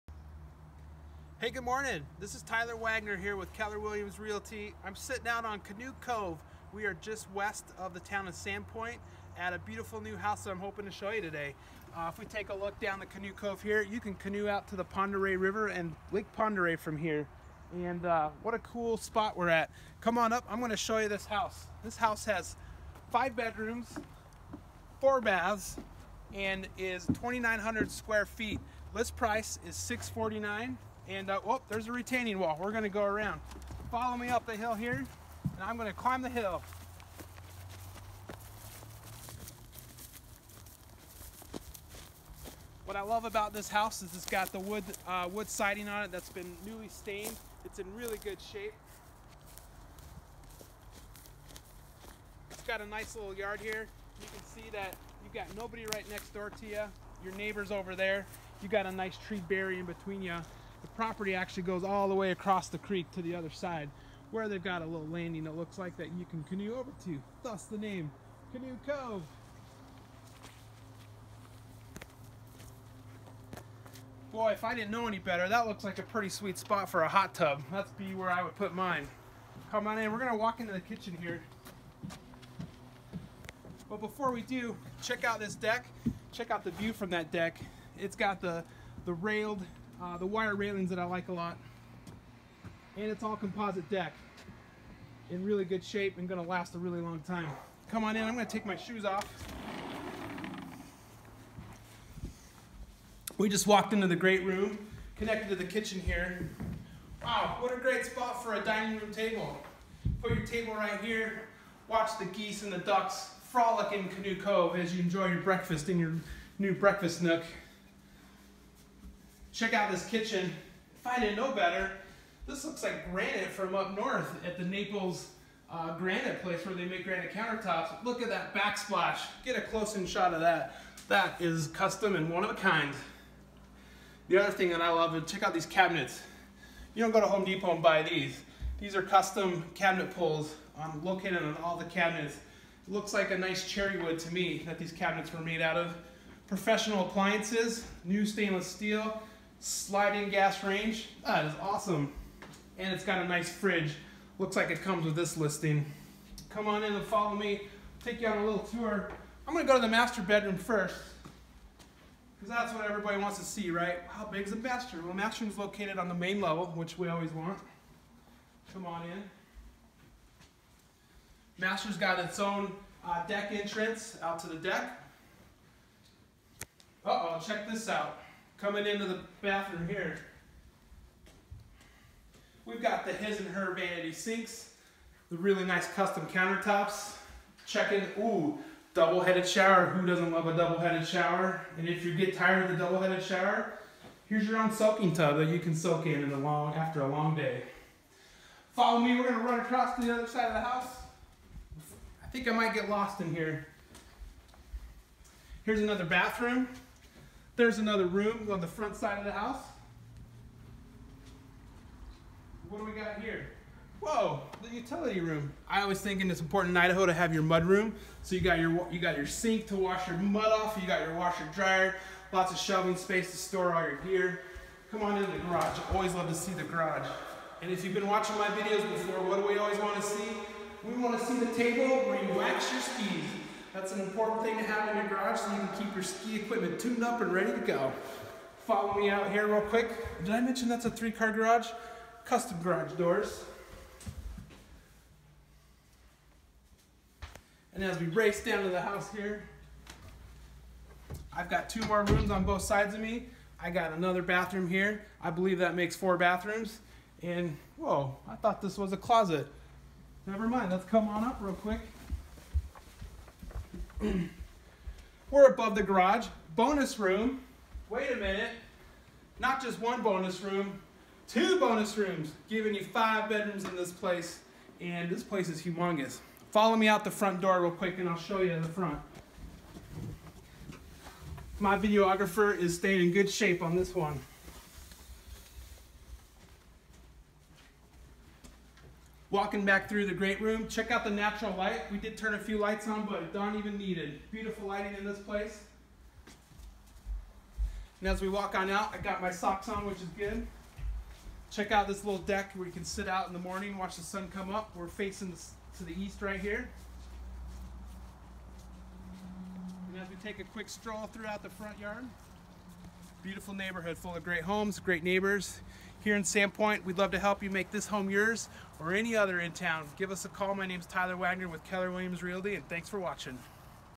Tyler Wagner Realtor presents 190 Canoe Cover - Sandpoint, Idaho 83864
Check out this video on a beautiful listing on Canoe Cover in Sandpopint Idaho.